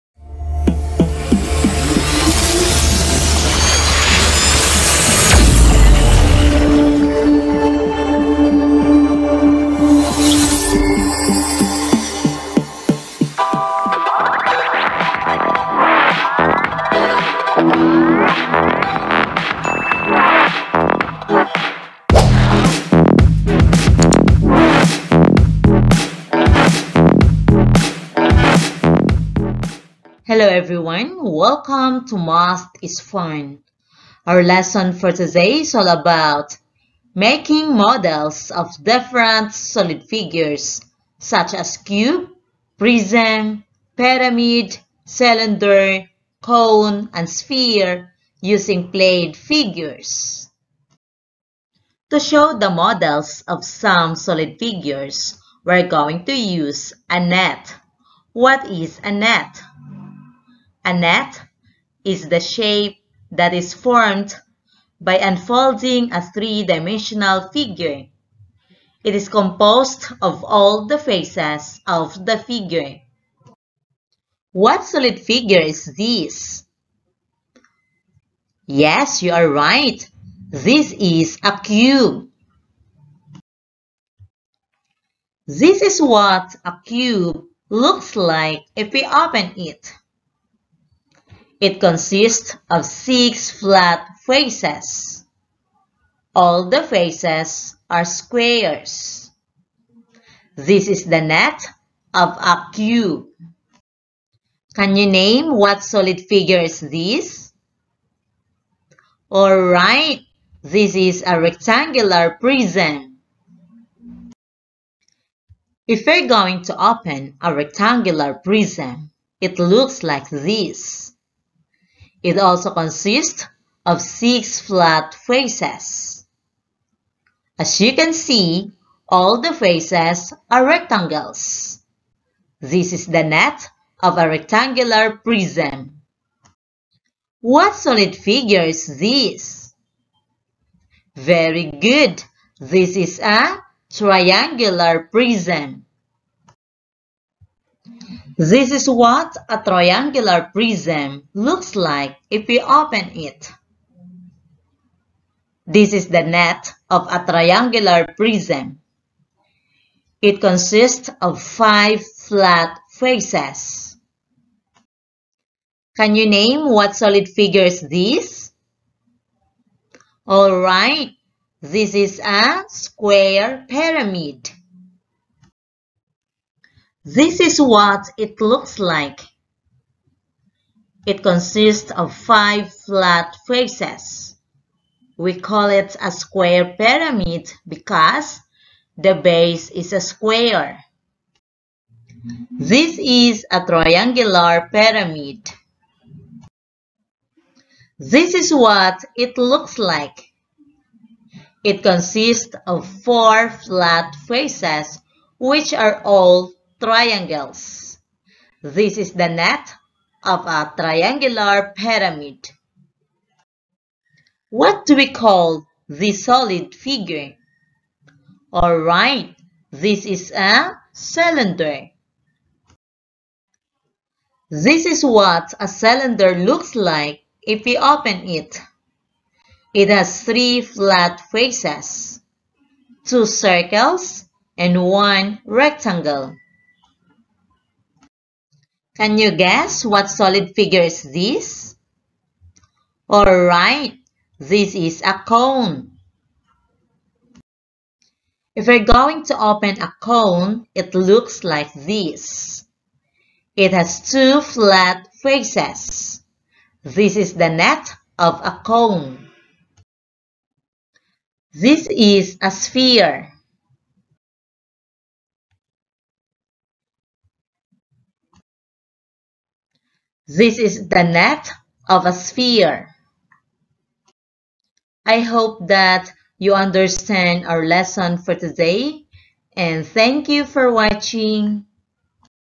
Making Models of Different Solid Figures: Cube, Prism, Pyramid, Cylinder, Cone, and Sphere
This video is all about making models of different solid figures: cube, prism, pyramid, cylinder, cone, and sphere using plane figures.